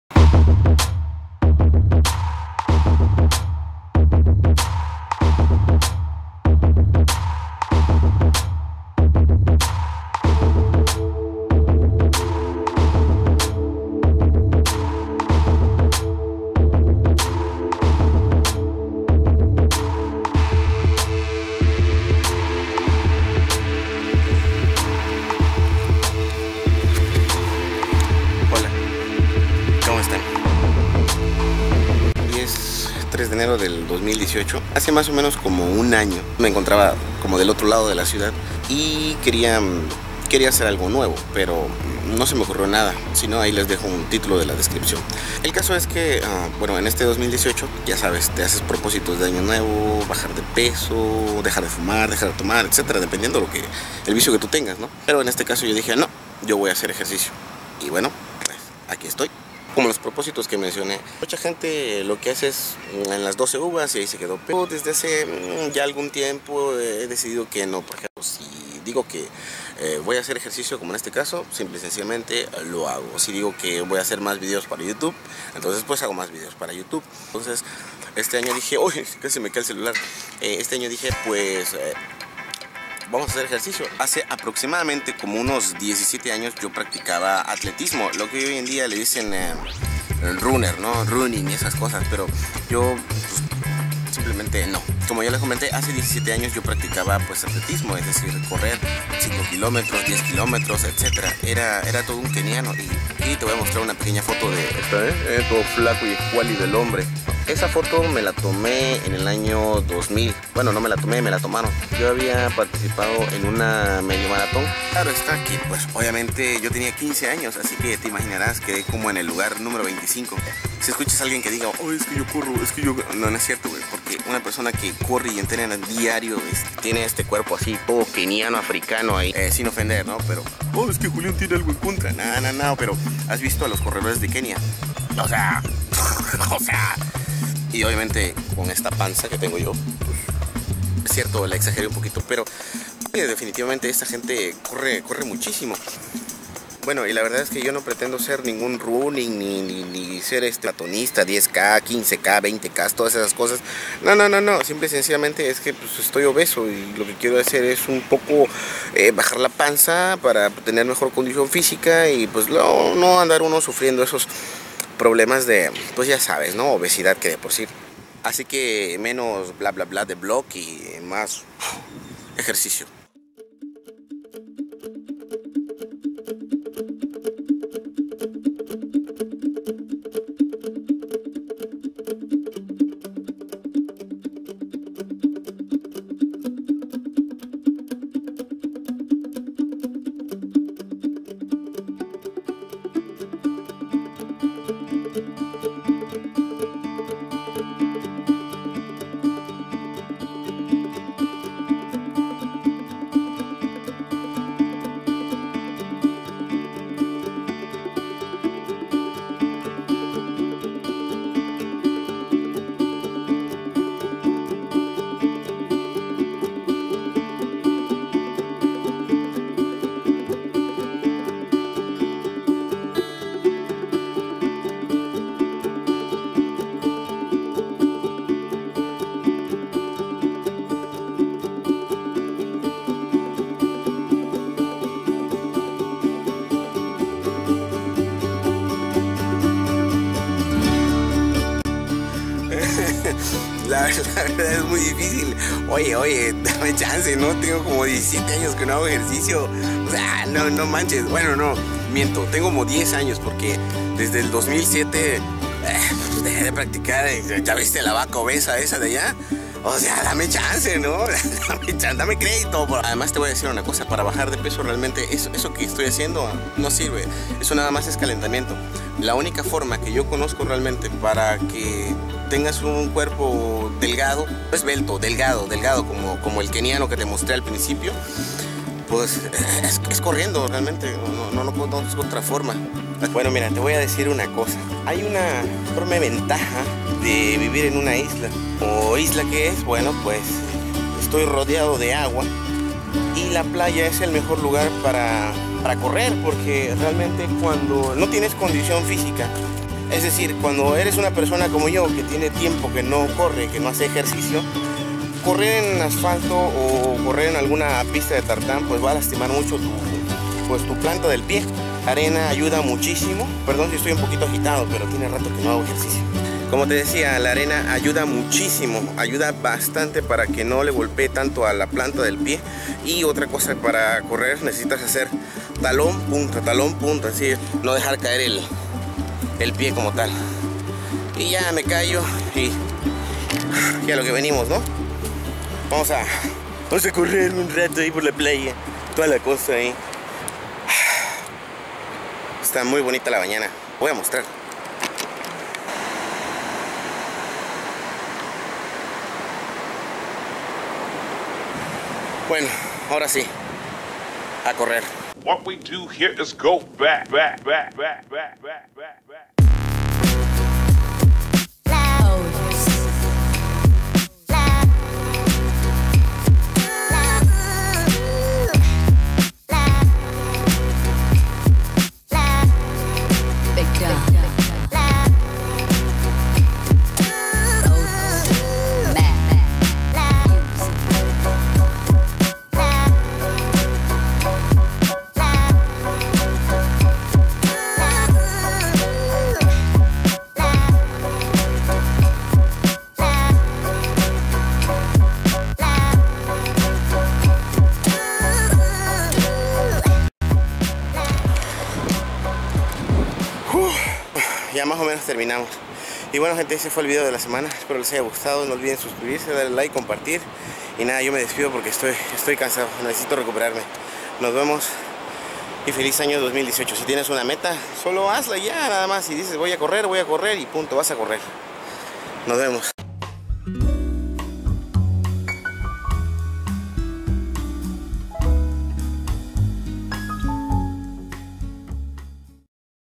Empezar a correr-bajar de peso-propósito de año nuevo.
Hola gente, les dejó un vídeo donde importa más la voluntad de hacer las cosas que los buenos deseos, esta actividad por las mañanas ayuda a bajar de peso.

Este vídeo habla de los cambios que necesitamos hacer.